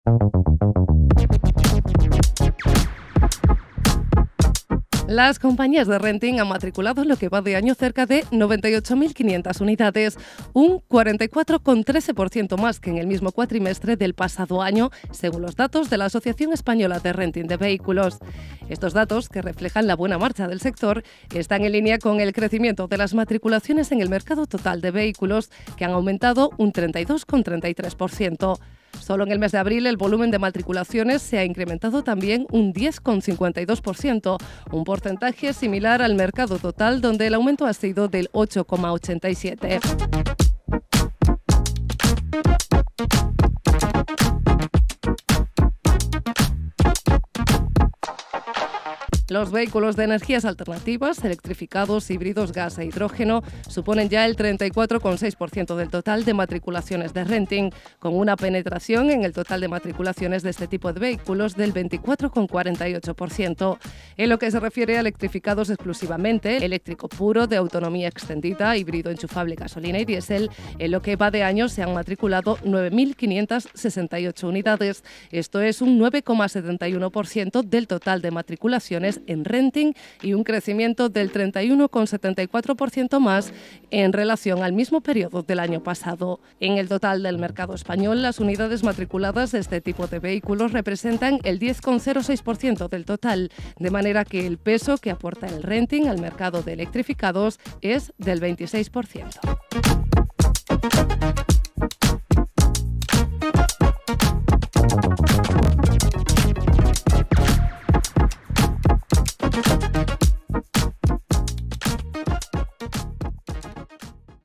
EL RENTING AUMENTA EN MÁS DE UN 44% SU VOLUMEN DE MATRICULACIONES HASTA ABRIL | By CdRas
Las compañías de renting han matriculado en lo que va de año cerca de 98.500 unidades, un 44,13% más que en el mismo cuatrimestre del pasado año, según datos de la Asociación Española de Renting de Vehículos. Estos datos, que reflejan la buena marcha del sector, están en línea con el crecimiento de las matriculaciones en el mercado total de vehículos, que han aumentado un 32,33. Solo en el mes de abril el volumen de matriculaciones se ha incrementado también un 10,52%, un porcentaje similar al del mercado total, donde el aumento ha sido de 8,87%.

Los vehículos de energías alternativas (electrificados, híbridos, gas e hidrógeno) suponen ya el 34,6% del total de matriculaciones de renting, con una penetración en el total de matriculaciones de este tipo de vehículos del 24,48%. En lo que se refiere a los electrificados exclusivamente (eléctrico puro, de autonomía extendida, híbrido enchufable gasolina y diésel), en lo que va de año se han matriculado 9.568 unidades, esto es un 9,71% del total de las matriculaciones en renting y un crecimiento del 31,74% más en relación al mismo periodo del pasado año. En el total del mercado español, las unidades matriculadas de este tipo de vehículos representan el 10,06% del total, de manera que el peso que aporta el renting al mercado de electrificados es del 26%.